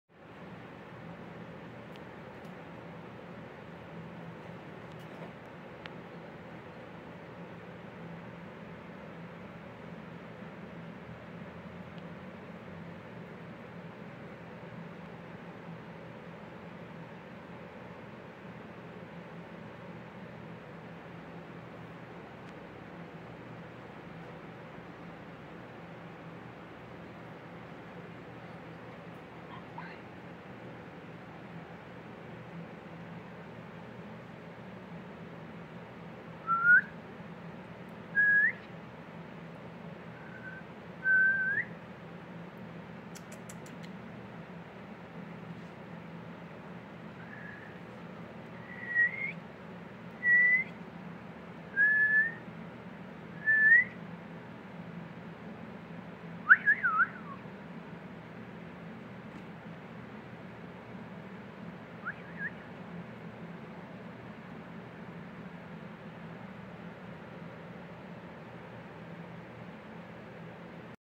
Que bello🐦💙 Periquito Australiano
Les presento a mi nuevo Hermoso periquito🐦 australiano, ahora coffe tiene alguien mas con quien jugar